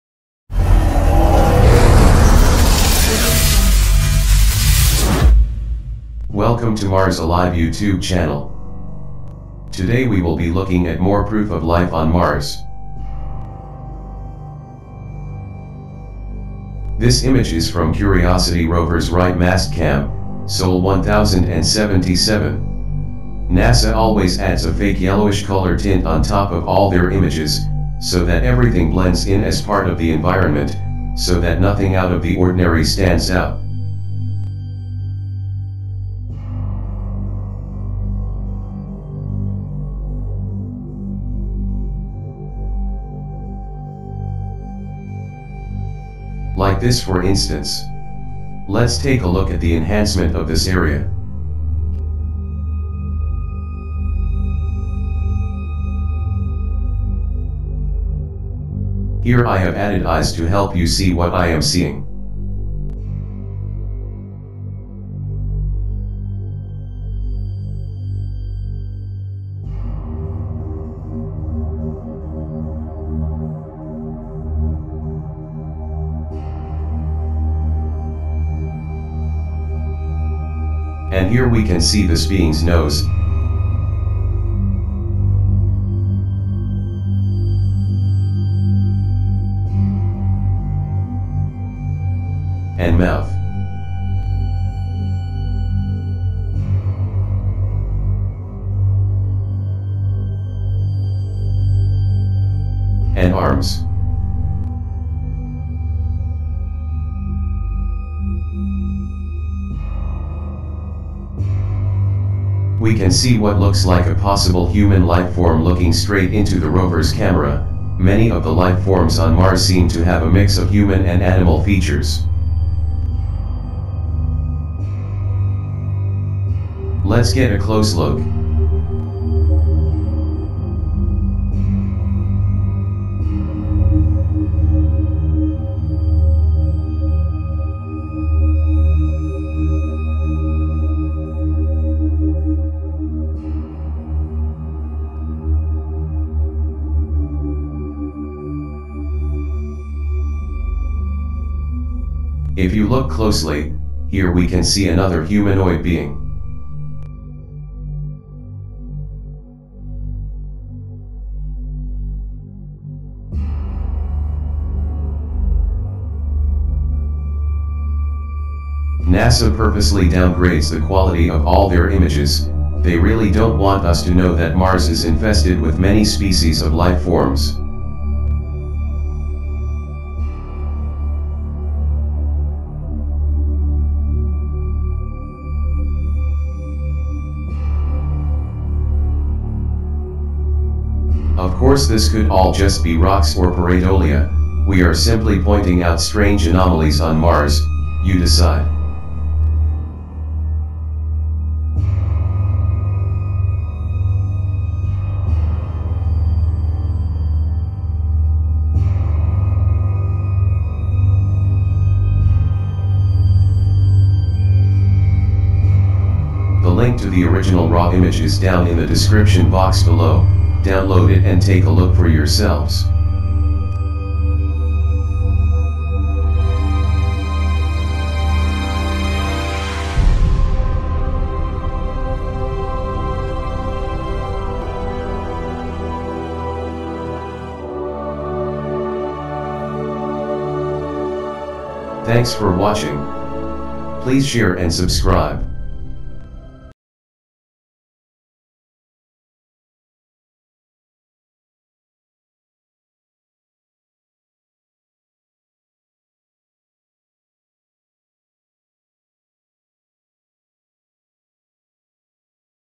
Life Form on Mars
MARS ALIVE - Searching for life on Mars!

This image is from  Curiosity Rover's right mastcam, Sol 1077.
In this image we can see what looks like a strange humanoid, many of the beings on Mars seem to have a mix of human and animal features.

Let me know what your thoughts are on this Mars Anomaly in the comment section below.